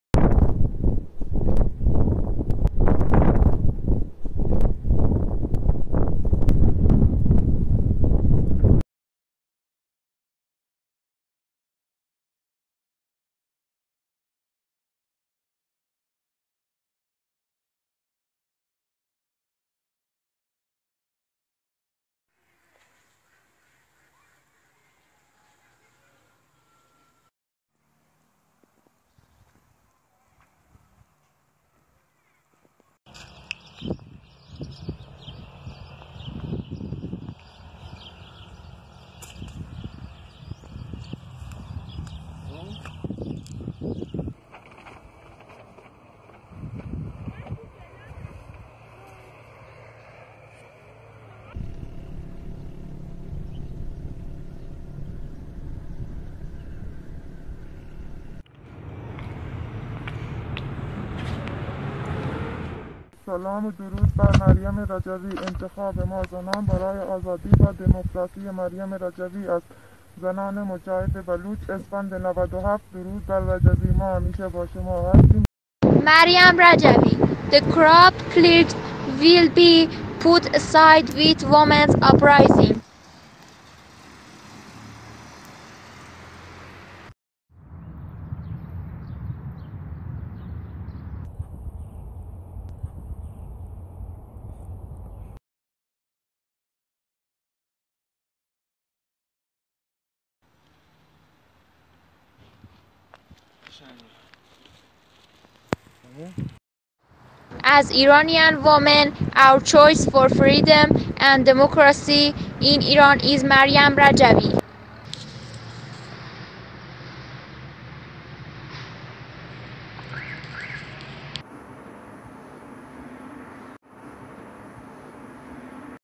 زنان ایران در خط مقدم نبرد با بنیادگرایی، فعالیت در کرمان، شهرکرد، بابل، اهواز، سمنان، زاهدان، ساری
اعضای کانون شورشی ۹۸۷ کرمان تراکت «مریم رجوی: آخوندهای فاسد با قیام زنان ایران جارو خواهند شد» را به‌ مناسبت ۸مارس برافراشتند.

در شهرکرد نیز تراکتهای حاوی تصویر و جمله‌های «مریم رجوی: من یک آرزو دارم هرکس آزاد باشد که پوشش و راه و رسم زندگیش را خودش انتخاب کند و زنان در همه امور از جمله رهبری جامعه از حقوق برابر با مردان برخودار باشند – نه به دین اجباری نه به پوشش اجباری نه به حکومت اجباری می‌توان و باید» توسط یکی از اعضای کانون‌های شورشی به‌ مناسبت ۸ مارس برافراشته شد.

تراکتهای «اکنون نوبت زنان است که علیه تمامی اشکال ستم بپا خیزند – نه حجاب اجباری نه دین اجباری نه حکومت اجباری» توسط اعضای کانون‌های شورشی در بابل برافراشته شد.

در اهواز نیز اعضای کانون شورشی مریم قدسی مآب به‌ مناسبت ۸ مارس، تراکت نه به دین اجباری نه به پوشش اجباری نه به حکومت اجباری را به زبان انگلیسی برافراشته کردند.

اعضای کانون شورشی ۹۸۷ سمنان نیز تراکتی که در آن از برنامه ۱۰ ماده‌ای مریم رجوی برای ایران آزاد فردا حمایت کرده بودند را به‌ مناسبت روز جهانی زن برافراشتند.

در زاهدان نیز یکی از زنان عضو کانون شورشی ضمن بلند کردن تراکتی که بر آن نوشته شده بود «سلام و درود بر مریم رجوی – انتخاب ما زنان برای آزادی و دموکراسی مریم رجوی است – ما همیشه با تو هستیم رجوی – زنان مجاهد بلوچ» گفت: درود بر رجوی ما همیشه با شما هستیم.

در ساری، تهران، خرم‌آباد، اصفهان، اهواز و لاهیجان نیز اعضای کانون‌های شورشی با بلند کردن تراکتهایی حاوی جملات مریم رجوی مانند «آخوندهای فاسد با قیام زنان ایران جارو خواهند شد – زنان قیام‌کننده که در بسیاری از صحنه‌ها پیشتاز بودند مایه افتخار ایران و ایرانی شدند – زنان ایرانی قدرت تغییر رژیم ملاها را دارند...» به زبانهای فارسی و انگلیسی روز جهانی زن را گرامی‌داشتند.